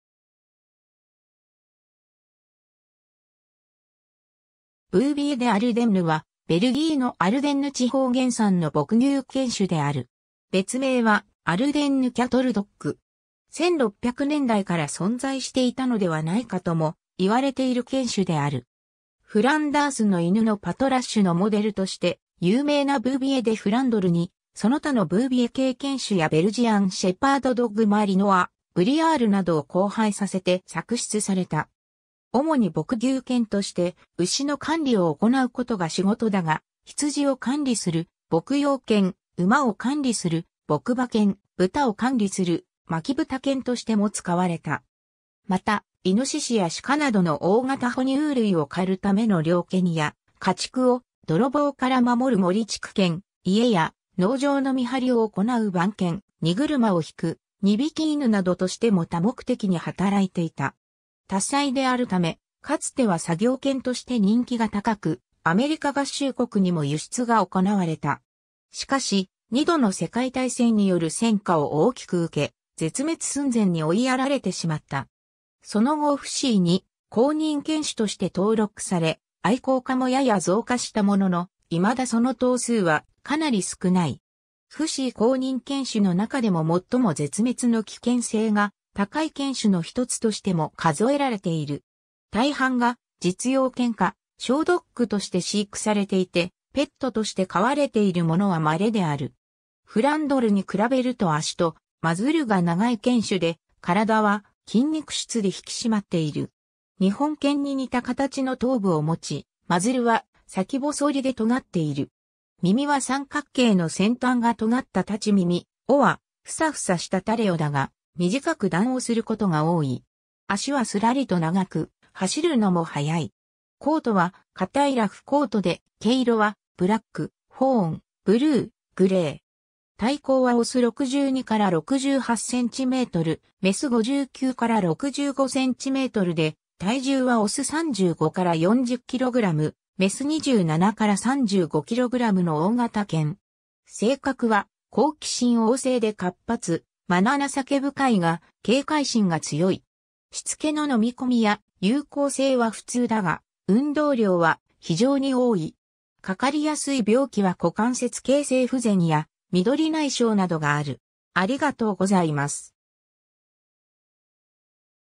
ブービエ・デ・アルデンヌ
ブービエ・デ・アルデンヌ（英：Bouvier des Ardennes）は、ベルギーのアルデンヌ地方原産の牧牛犬種である。
別名はアルデンヌ・キャトル・ドッグ（英：Aldennes Cattle Dog）。
 1600年代から存在していたのではないかとも言われている犬種である。
『フランダースの犬』のパトラッシュのモデルとして有名なブービエ・デ・フランドルに、その他のブービエ系犬種（どの種類かははっきりしない）やベルジアン・シェパード・ドッグ・マリノア、ブリアールなどを交配させて作出された。
 主に牧牛犬として牛の管理を行うことが仕事だが、羊を管理する牧羊犬、馬を管理する牧馬犬（ぼくばけん）、豚を管理する牧豚犬（ぼくとんけん）としても使われた。
又、猪や鹿などの大型哺乳類を狩るための猟犬や、家畜を泥棒から守る護蓄犬、家や農場の見張りを行う番犬、荷車を引く荷引き犬などとしても多目的に働いていた。
 多才であるため、かつては作業犬として人気が高く、アメリカ合衆国にも輸出が行われた。
然し、2度の世界大戦による戦禍を大きく受け、絶滅寸前に追いやられてしまった。
その後FCIに公認犬種として登録され、愛好家もやや増加したものの、いまだその頭数はかなり少ない。
FCI公認犬種の中でも最も絶滅の危険性が高い犬種のひとつとしても数えられている。
大半が実用犬かショードッグとして飼育されていて、ペットとして飼われているものはまれである。
 フランドルに比べると脚とマズルが長い犬種で、体は筋肉質で引き締まっている。
日本犬に似た形の頭部を持ち、マズルは先細りで尖っている。
耳は三角形の先端が尖った立ち耳、尾はふさふさした垂れ尾だが、短く断尾することが多い。
脚はすらりと長く、走るのも速い。
コートは硬いラフコートで、毛色はブラック、フォーン、ブルー、グレー。
体高は雄62〜68cm、雌59〜65cmで体重は雄35〜40kg、雌27〜35kgの大型犬。
性格は好奇心旺盛で活発、愛情深いが警戒心が強い。
しつけの飲み込みや友好性は普通だが、運動量は非常に多い。
かかりやすい病気は股関節形成不全や緑内障などがある。